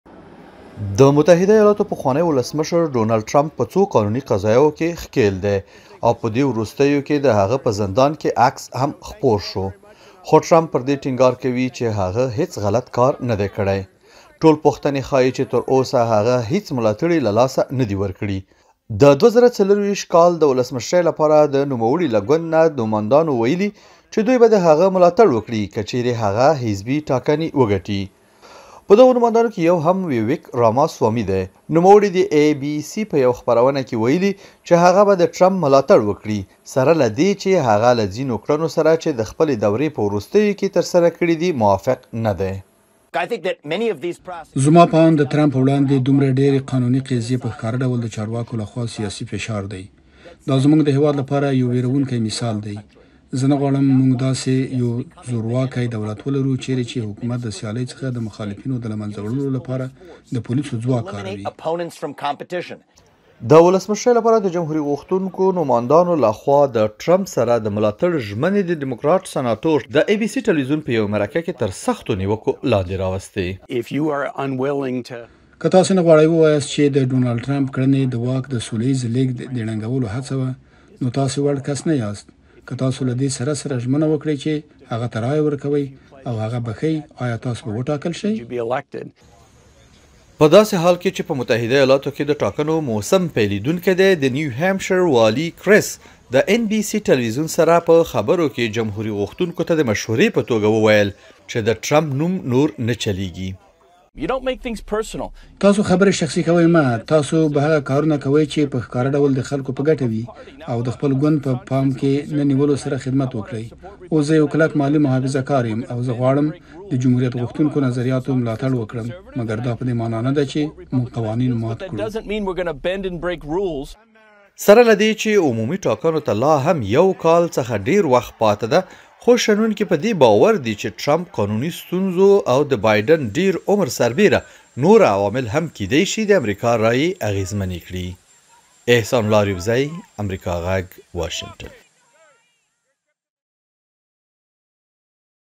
د متحده ایالاتو راتلونکو ولسشمرۍ ټاکنو ته کتنه
د متحده ایالاتو د اوسني ولسمشر جو بایډن او د هغه مخکیني سیال پخواني ولسمشر دونالډ ټرمپ تر منځ په ټاکنو کې  د سیالیو امکانات خورا لوړ ښکاري. دا په داسې حال کې ده چې نور سیاستوال هم په دغو سیالیو کې د ګډون چانس لري.